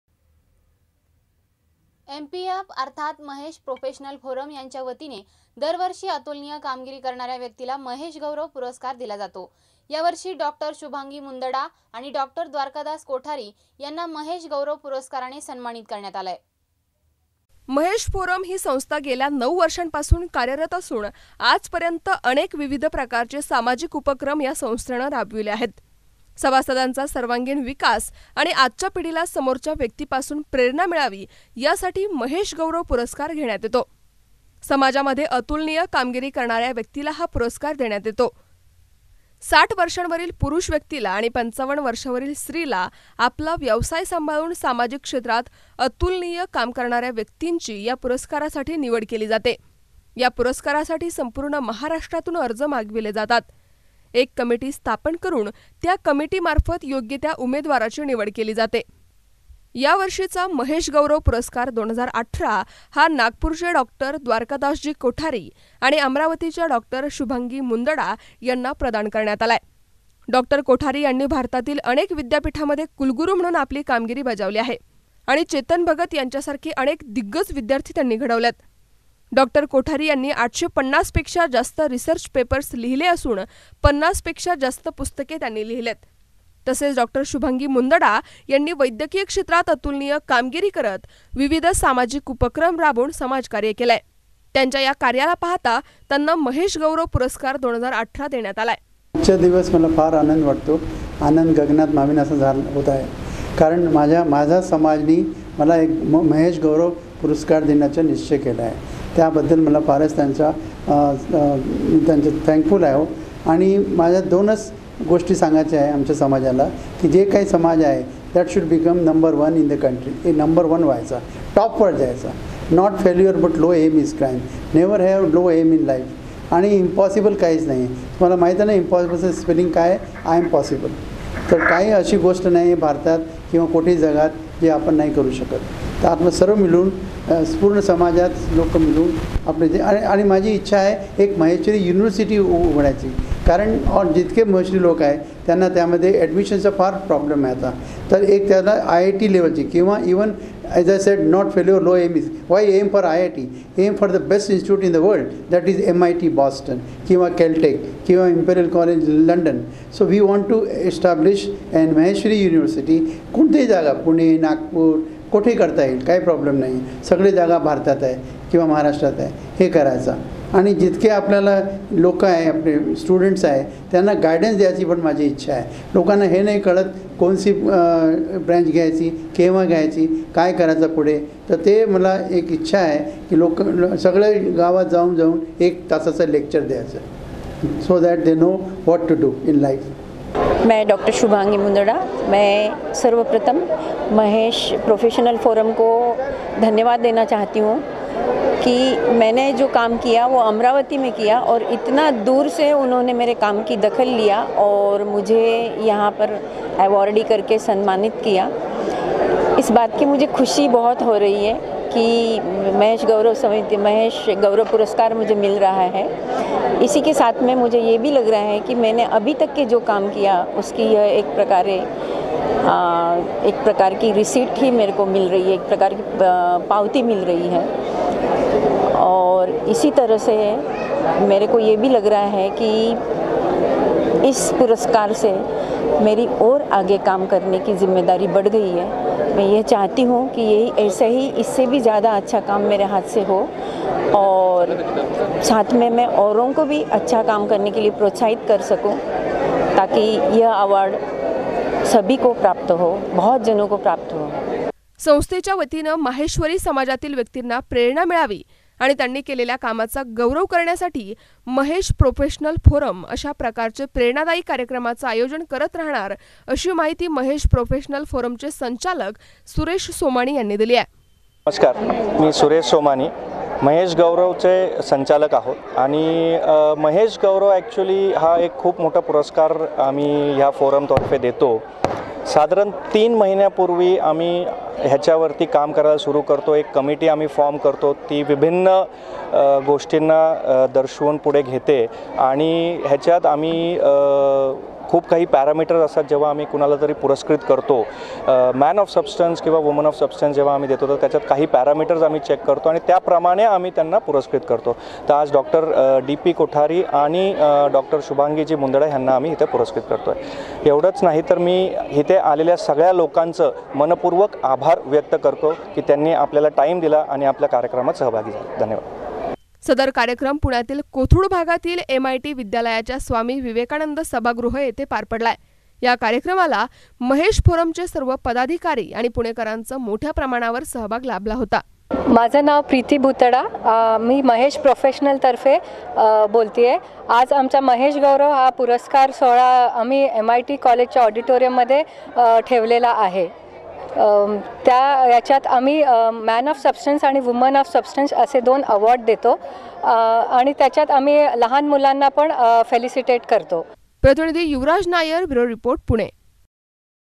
महेश गौरव पुरस्कार वितरण, डॉ. शुभांगी मुंदडा आणि डॉ. द्वारकादास कोठारी यांना पुरस्कार प्रदान
एम. पी. एफ. अर्थात महेश प्रोफेशनल फोरम यांच्या वतीने दरवर्षी अतुलनीय कामगिरी करणाऱ्या व्यक्तीला महेश गौरव पुरस्कार दिला जातो, या वर्षी डॉक्टर शुभांगी मुंदडा आणि डॉक्टर द्वारकादास कोठारी यांना महेश गौरव पुरस्काराने सन्मानित करण्यात आलय